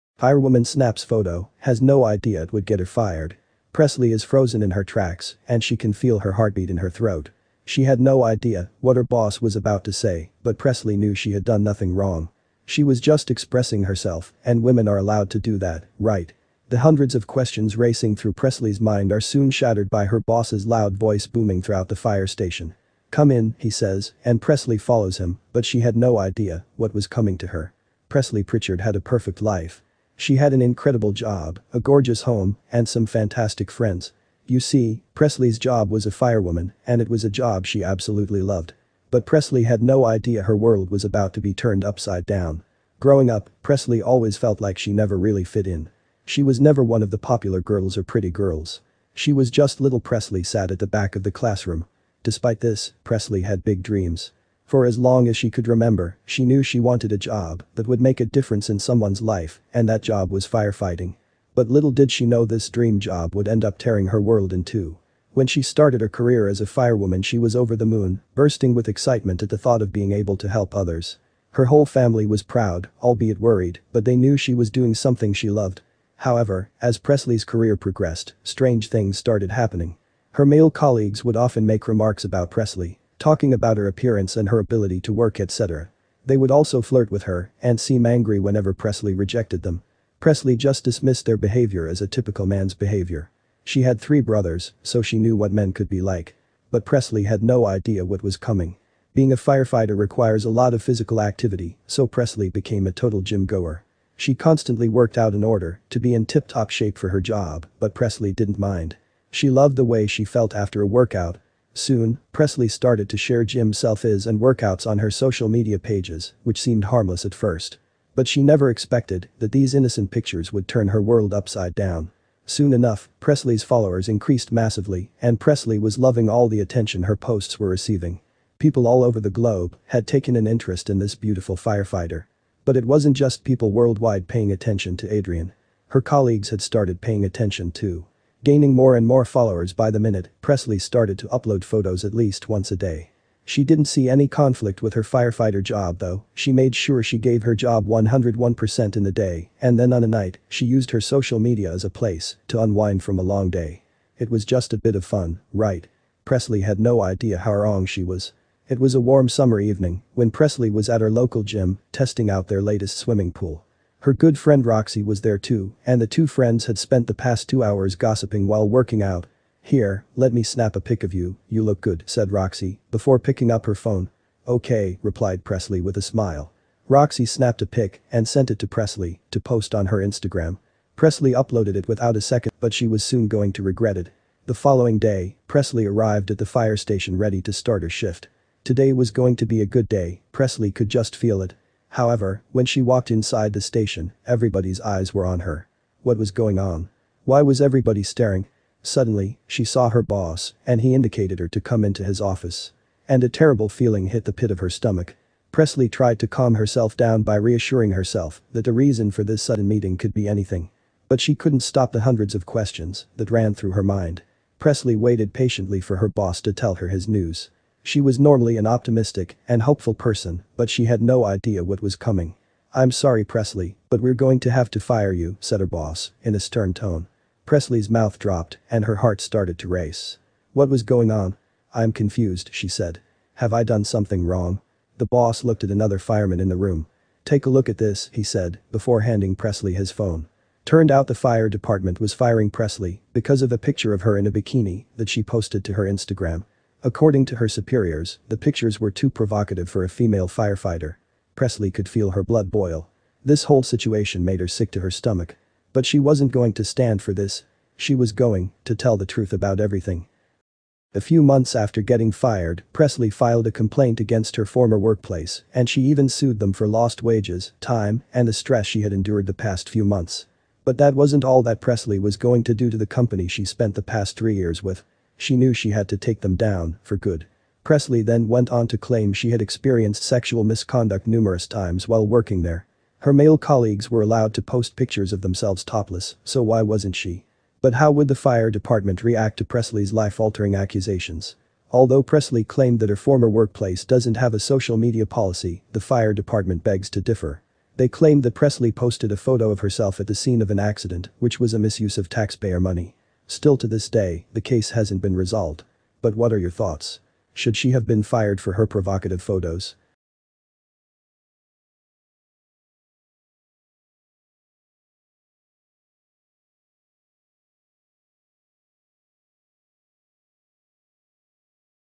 Firewoman Snaps Photo, Has No Idea It Would Get Her Fired
Presley is frozen in her tracks and she can feel her heartbeat in her throat. She had no idea what her boss was about to say, but Presley knew she had done nothing wrong. She was just expressing herself, and women are allowed to do that, right? The hundreds of questions racing through Presley’s mind are soon shattered by her boss’s loud voice booming throughout the fire station. “Come in,” he says, and Presley follows him, but she had no idea what was coming to her.